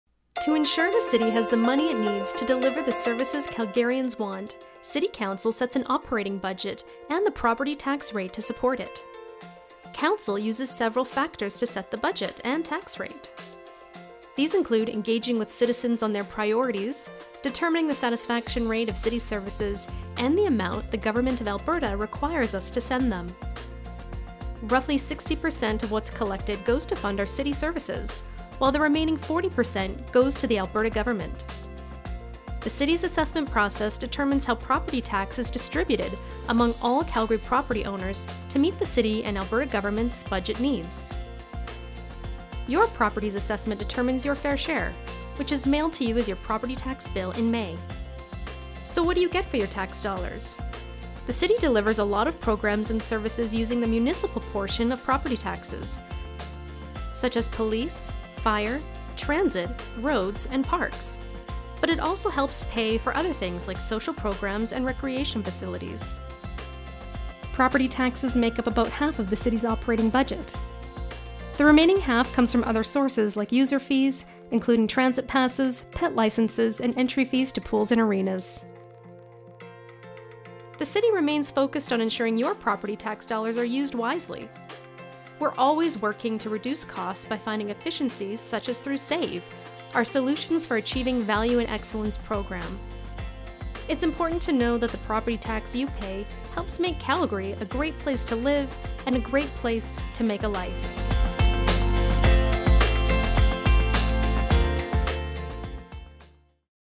Calgary's property tax dollars at work
Many Calgarians have told us that you want to know more about how your tax dollars are invested. Here's how it works:

To ensure we have the money we need to deliver the services Calgarians want, City Council sets an operating budget and the property tax rate to support it.

Council uses several factors to set the budget and tax rate; these include engaging with citizens on their priorities, determining the satisfaction rate of City services, and the amount the Government of Alberta requires us to send them.

Roughly 60 per cent of what's collected goes to fund our City services, while the remaining 40 percent goes to the Government of Alberta.

Our assessment process determines how property tax is distributed among all Calgary property owners to meet The City and Alberta Government's budget needs. Your property's assessment determines your fair share, which is mailed to you as your property tax bill in May.

So what do you get for your tax dollars? We deliver a lot of programs and services using the municipal portion of property taxes, such as firefighting, public transit, roads and parks; but it also helps pay for other things, like social programs and recreation facilities.

Property taxes make up about half of our operating budget. The remaining half comes from other sources, like user fees. This includes transit passes, pet licenses and entry fees to pools and arenas.

We remain focused on ensuring your property tax dollars are used wisely. We're always working to reduce costs by finding efficiencies. It's important to know that the property tax you pay helps to make Calgary a great place to live; and a great place to make a life.

We invite you to learn more about The City’s finances by visiting the “Our Finances” playlist.